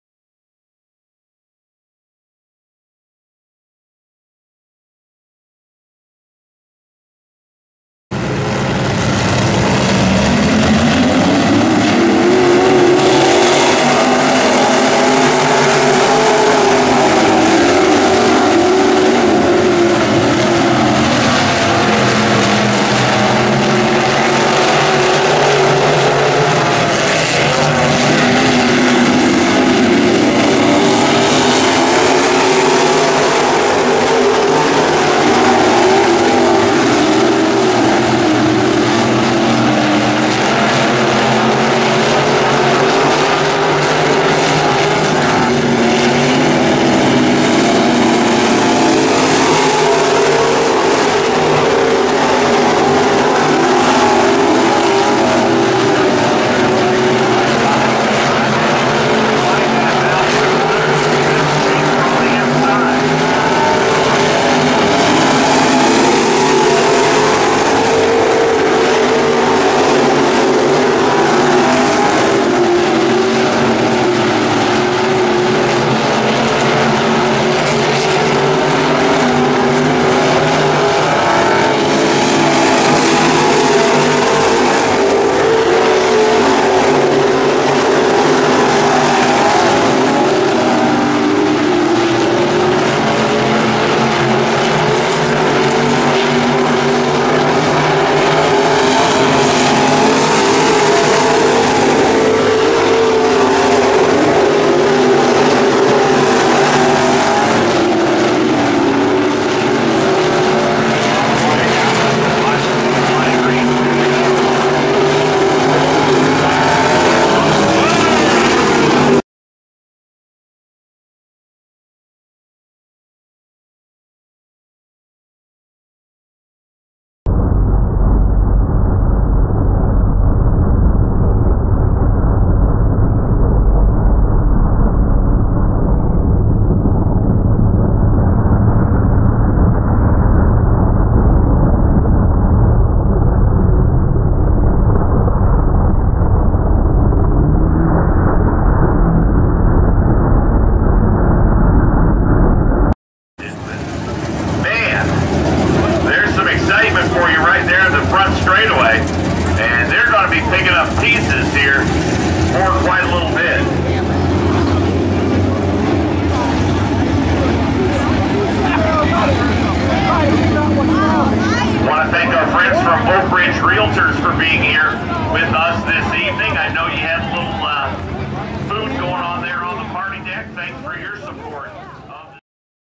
Kevin Hurst IMCA Sportmod heat race.
At Independence, Iowa. July 30, 2016. FAIL. CRASH.